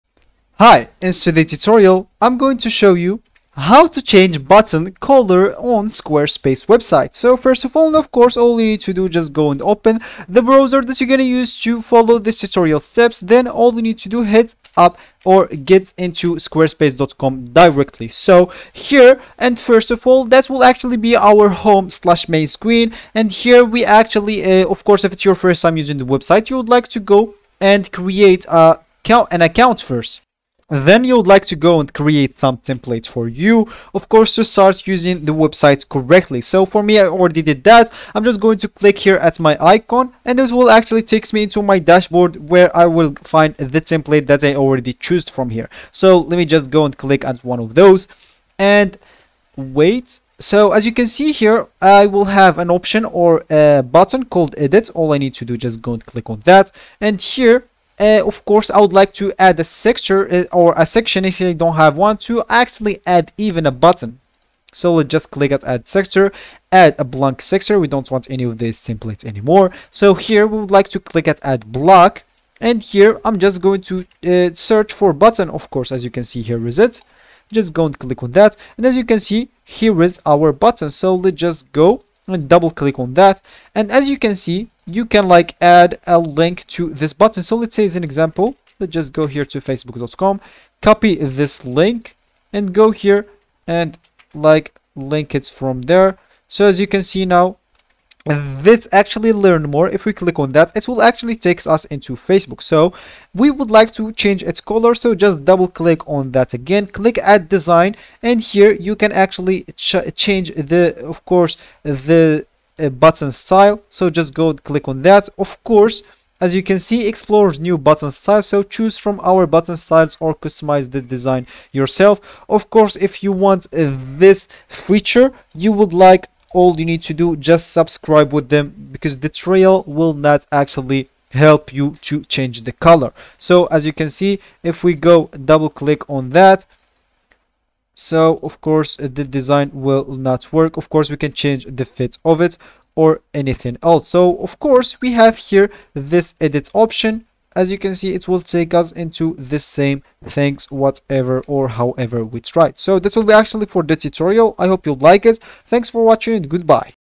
How To Change Button Color In Squarespace Tutorial
Today I show how to change button color in squarespace tutorial,squarespace tutorial,squarespace buttons,squarespace change button looks